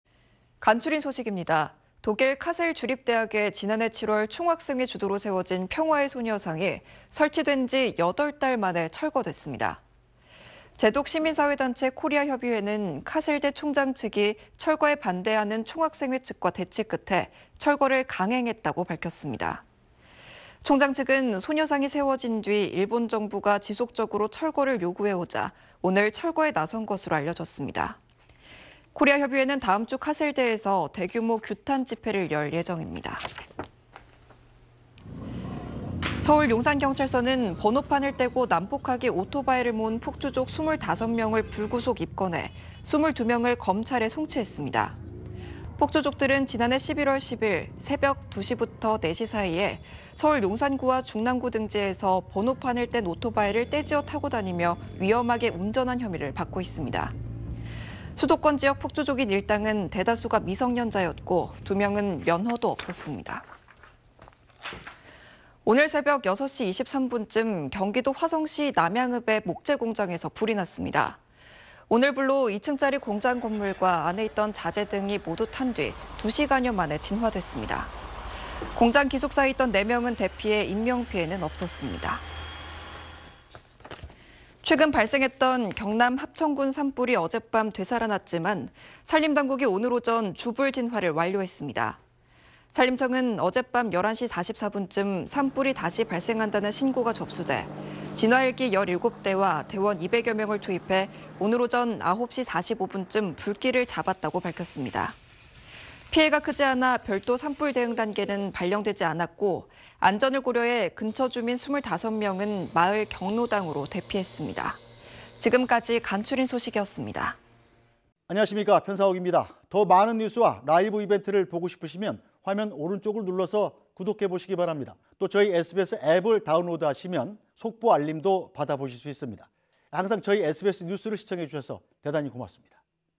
독일 카셀대 소녀상, 설치 8개월 만에 기습 철거 / SBS
독일 카셀 주립대학에 지난해 7월 총학생회 주도로 세워진 평화의 소녀상이 설치된 지 여덟 달 만에 철거됐습니다.

재독 시민사회단체 코리아협의회는 카셀대 총장 측이 철거에 반대하는 총학생회 측과 대치 끝에 철거를 강행했다고 밝혔습니다.

총장 측은 소녀상이 세워진 뒤 일본 정부가 지속적으로 철거를 요구해 오자 오늘(10일) 철거에 나선 것으로 알려졌습니다.

코리아협의회는 다음 주 카셀대에서 대규모 규탄 집회를 열 예정입니다.

서울 용산경찰서는 번호판을 떼고 난폭하게 오토바이를 몬 폭주족 25명을 불구속 입건해 22명을 검찰에 송치했습니다.

폭주족들은 지난해 11월 10일 새벽 2~4시 사이에 서울 용산구와 중랑구 등지에서 번호판을 뗀 오토바이를 떼 지어 타고 다니며 위험하게 운전한 혐의를 받고 있습니다.

수도권 지역 폭주족인 일당은 대다수가 미성년자였고 2명은 면허도 없었습니다.

오늘 새벽 6시 23분쯤 경기도 화성시 남양읍의 목재 공장에서 불이 났습니다.

오늘 불로 2층짜리 공장 건물과 안에 있던 자재 등이 모두 태운 뒤 2시간여 만에 진화됐습니다.

공장 기숙사에 있던 4명은 대피해 인명피해는 없었습니다.

최근 발생했던 경남 합천군 산불이 어젯밤 되살아났지만, 산림 당국이 오늘 오전 주불 진화를 완료했습니다.

산림청은 어젯밤 11시 44분쯤 산불이 다시 발생한다는 신고가 접수돼 진화 헬기 17대와 대원 200여 명을 투입해 오늘 오전 9시 45분쯤 불길을 잡았다고 밝혔습니다.

피해가 크지 않아 별도 산불 대응 단계는 발령되지 않았고, 안전을 고려해 근처 주민 25명은 마을 경로당으로 대피했습니다.